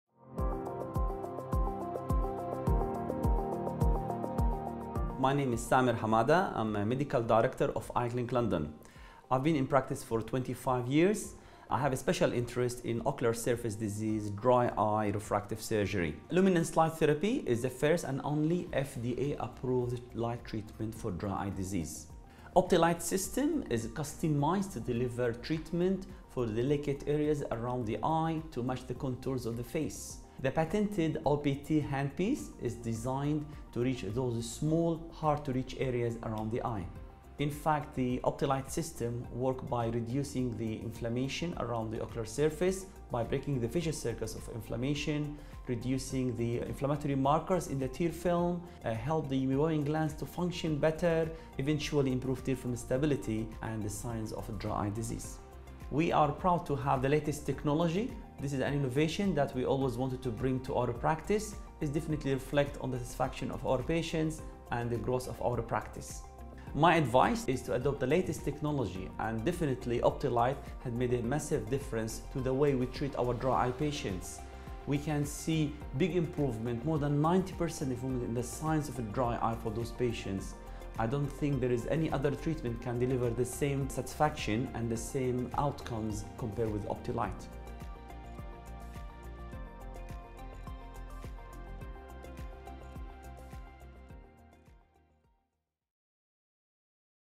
Introducing OptiLight by Lumenis and its features by Ophthalmologist Samer Hamada
Dr. Samar Hamada, a medical director at Eye Clinic London, shares his experience using OptiLIGHT for dry eye management and how it helps grow the practice.